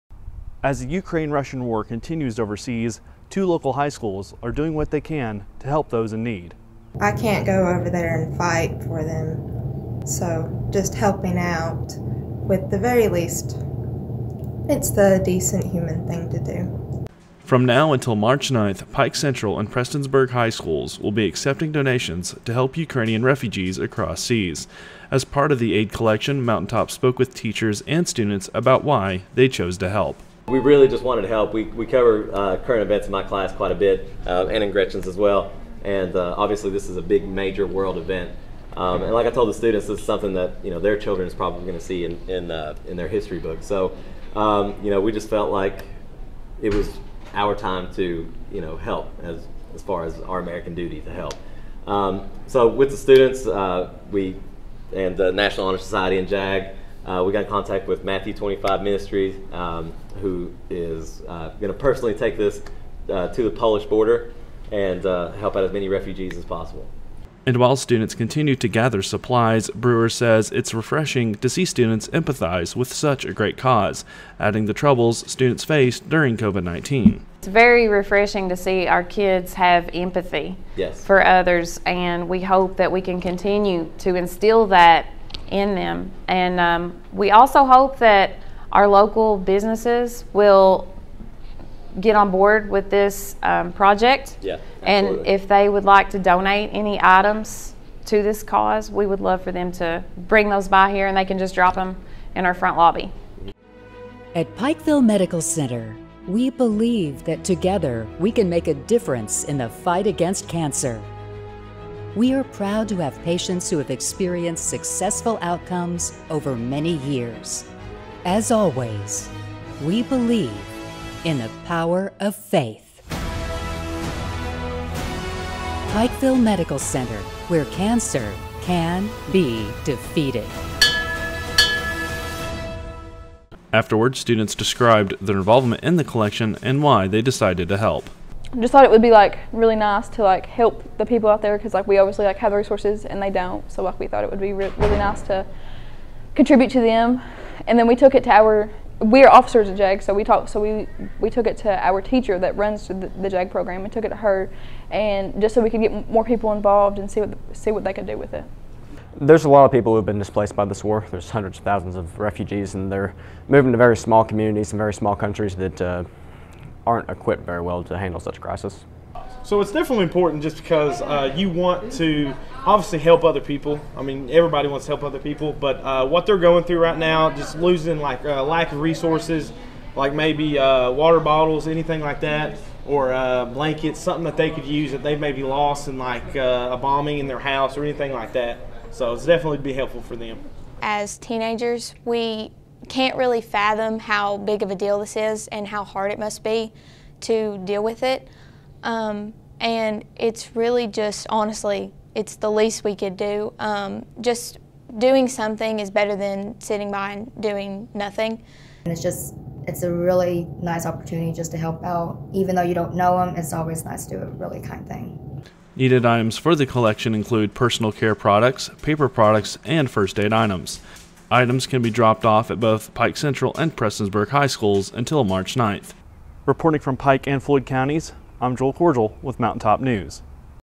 Local High School Students Help Ukrainian Refugees
From now until March 9th, Pike Central and Prestonsburg High Schools will be accepting donations to help Ukrainian refugees across the sea.